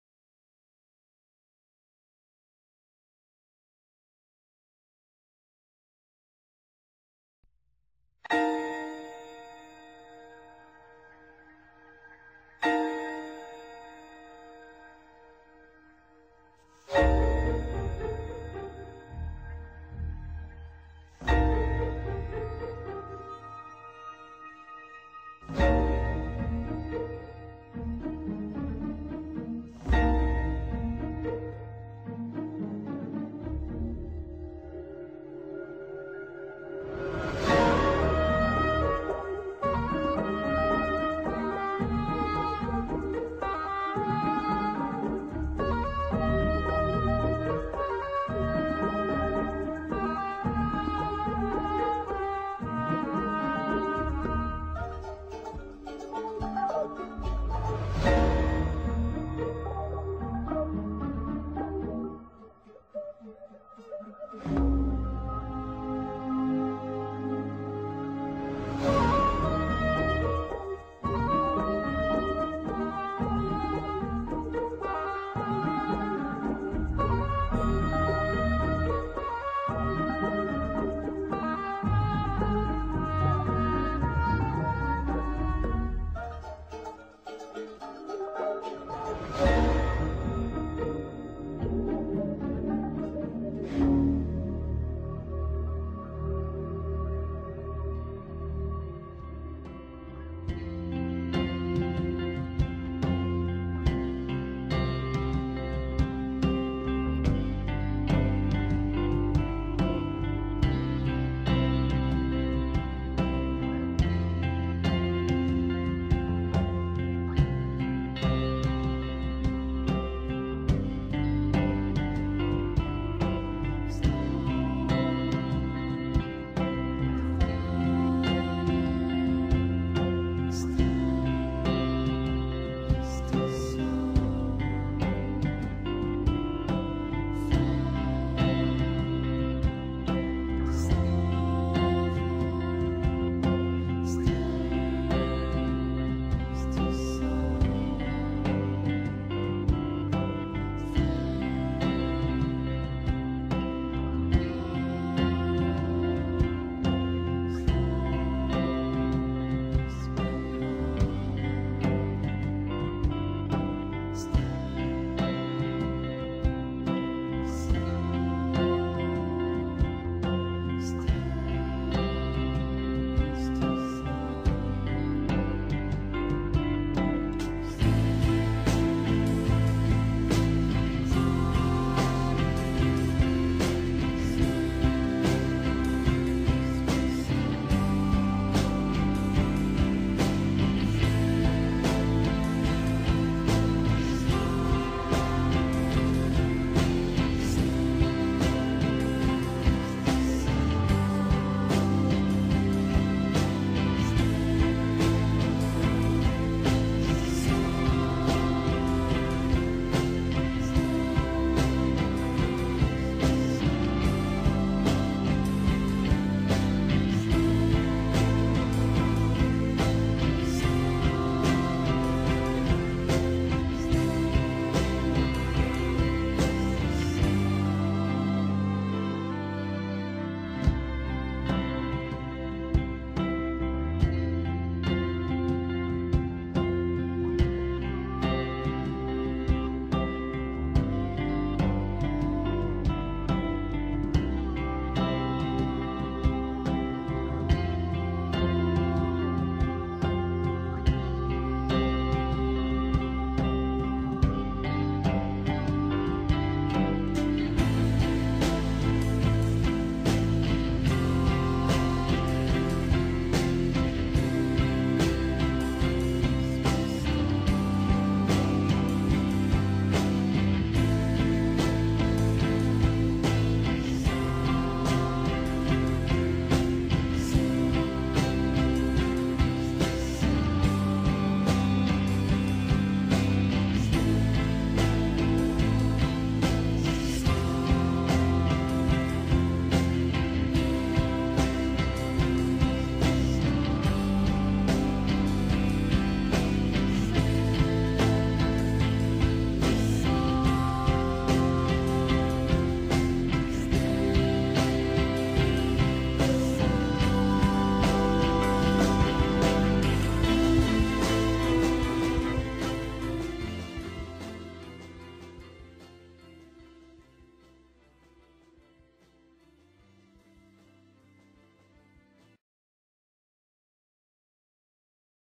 Set Design for animation - Minshar animation department year 2
Environment and set design by students at minshar for art animation and film school.